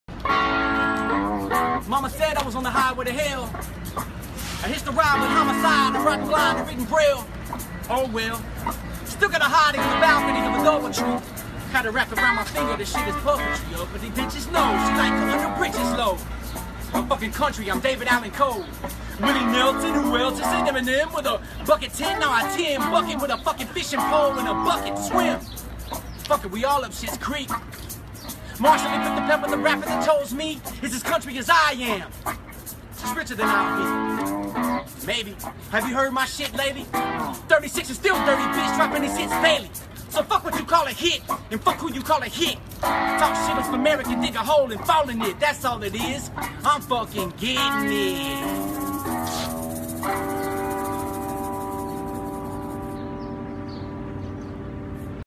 Yelawolf - Freestyle in Austin, TX (02.06.2016)
ePro Editorial Team:
NK
Kirill Bursov
Lilit Sagatelyan
Katerina Malik
Alexey Zakharenkov

Mamma said I was on the high to hell
I hitched a ride with homicide
I'm drivin blind and readin braille
Oh well
Still gotta hottie on the balcony of a Double Tree
Had her wrapped around my finger this shit is puppetry , uppiddy bitches no , she like cuttin her britches low
I'm fuckin country I'm David Allan Coe
Willie Nelson , who else s in ?
Eminem with a bucket tin
Nah , a tin bucket with a fuckin fishin pole in a bucket swim
Fuck it we all up shits creek
Marshall he picked a pepper the rapper who chose me
Is as country as I am
Just richer than I'll be Maybe
Have you heard my shit lately ?
36 and still dirty bitch droppin these hits daily
So fuck what you call a hit
And fuck who you call a hick
Talk shit on SLUMERICAN dig a hole and fall in it
That's all it is
I'm
@bonesowens @kleversworld SLUMERICAN SLUMERICANMADE ⚡️⚡️FUCKINGETTINIT⚡️⚡️ @shadyrecords in Austin TX where it all began ... @xgames @monstermusic @harleydavidson halleerrrrrrr 🎥 @deester35 !!